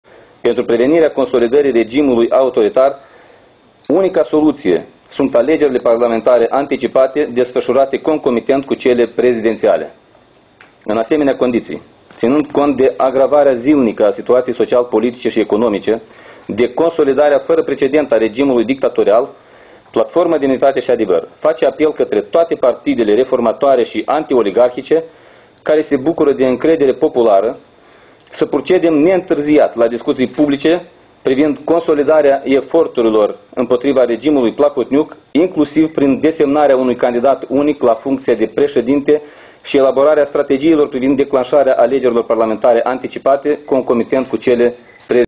Briefing susținut de Președintele Platformei Demitate și Adevăr, Andrei Năstase,...
Andrei Năstase: Pentru prevenirea consolidării regimului autoritar, unica soluție sunt alegerile parlamentare anticipate desfășurate concomitent cu alegerile prezidențiale
Titlul evenimentului: Briefing susținut de Președintele Platformei Demitate și Adevăr, Andrei Năstase, cu tema „Poziția Platformei Demnitate și Adevăr privind desemnarea candidatului unic al opoziției la funcția de președinte”